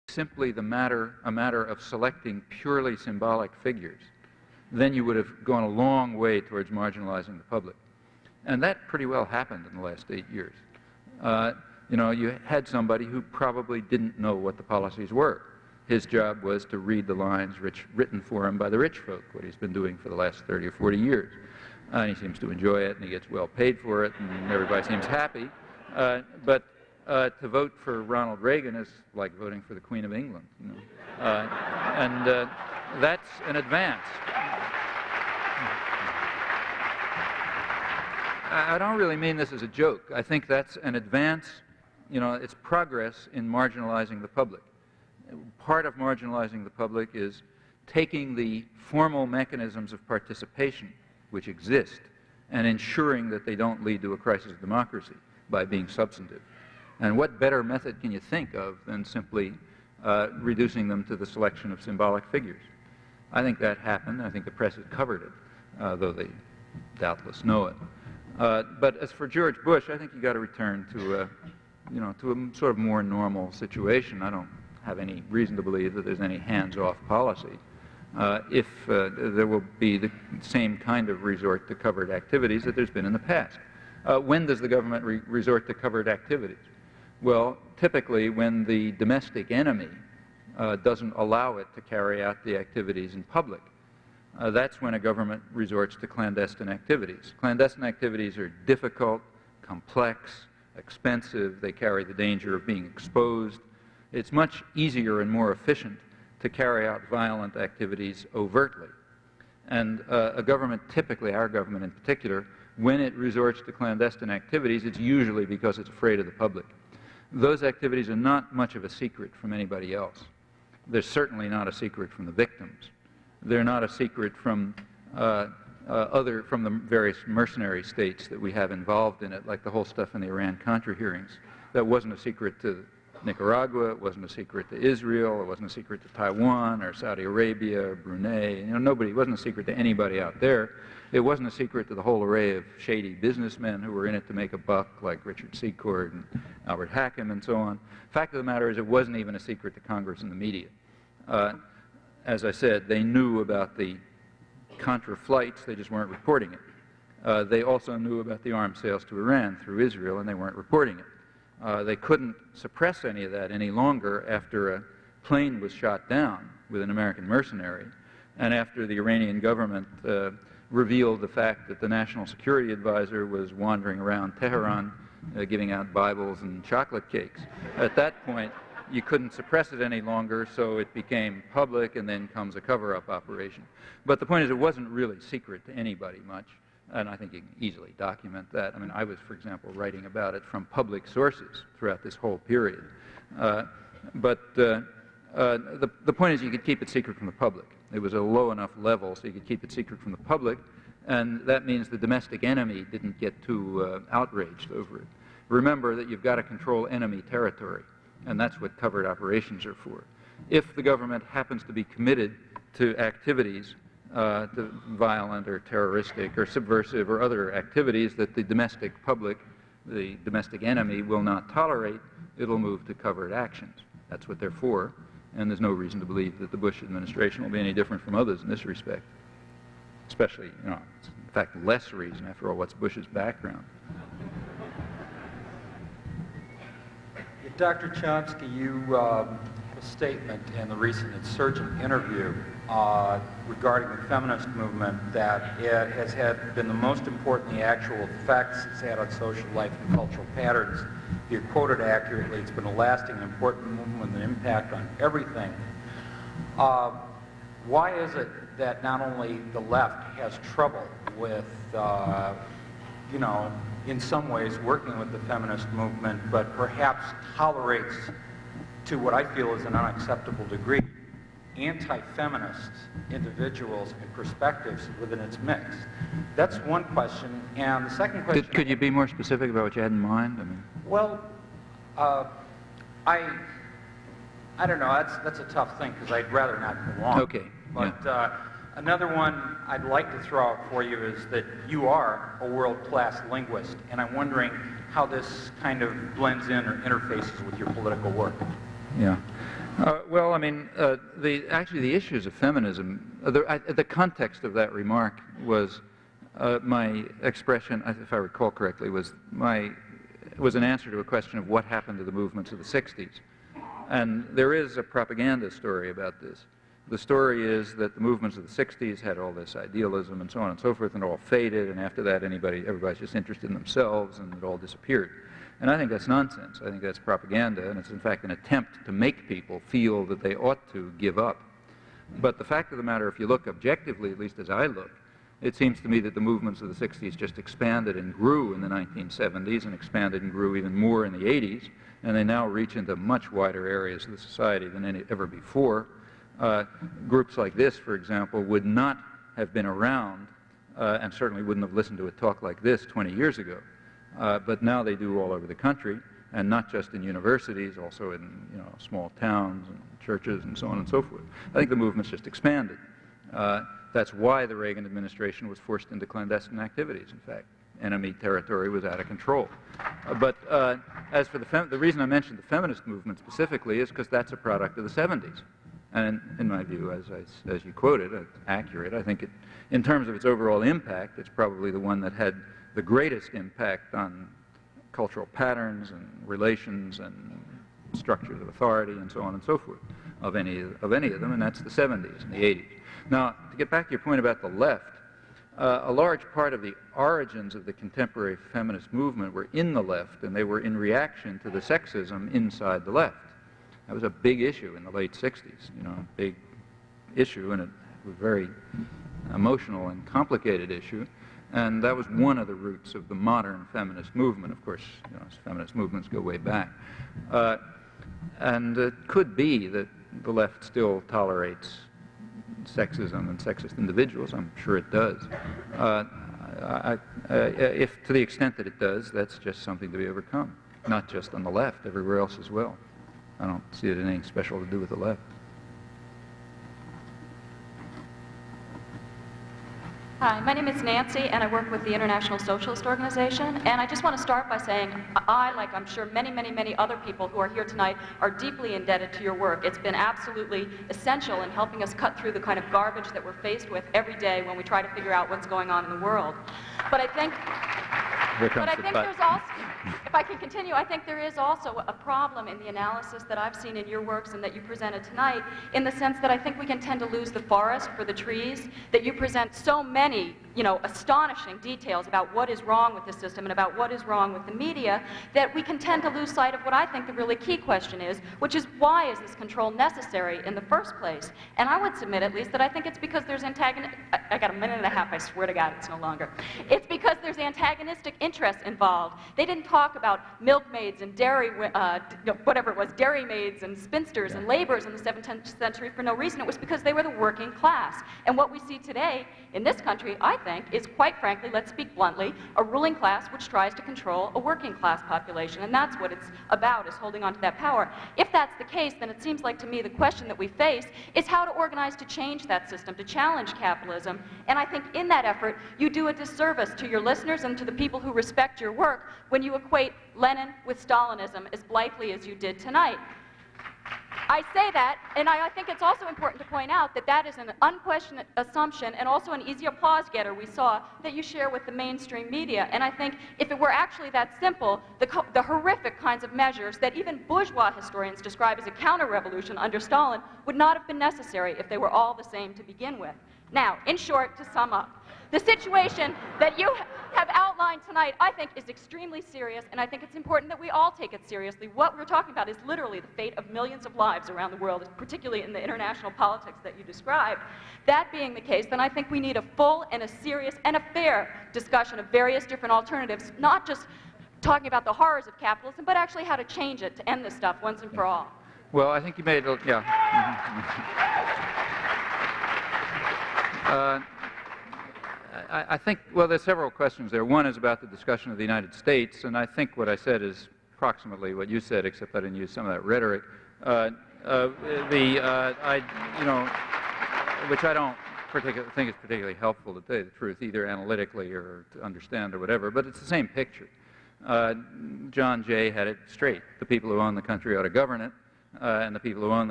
Q&A: Political Econ. of the Mass Media-from pdxjustice 2/8
Thanks to pdxjustice Media Production.

Credits:
Camera and Sound:

Steve Braker--Worthwhile Films
Rosemary Bodolay--Art Industry

Editing:

William Seaman-- pdxjustice Media Productions

The question and and answer session after the lecture by Noam Chomsky entitled "Manufacturing Consent: The Political Economy of the Mass Media."  at the Wisconsin Union Theater, on the University of Wisconsin campus in Madison, Wisconsin.